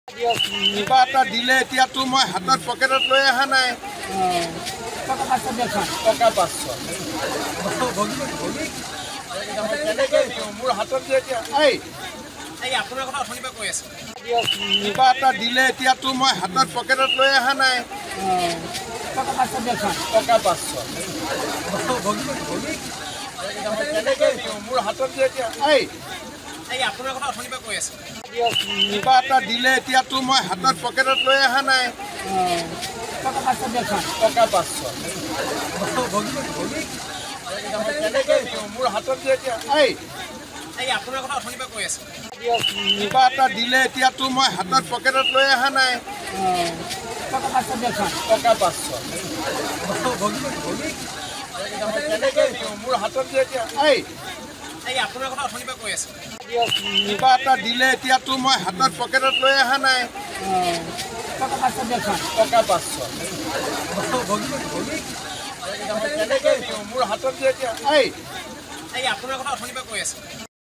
CM In Majuli | মাজুলীত বান পৰিদৰ্শনকালত মুখ্যমন্ত্ৰীক আৰ্থিক সহায় বিচাৰিলে এজন যুৱকে | N18V
লগতে চাওঁক দিনটোৰ বহু গুৰুত্বপূৰ্ণ খবৰ |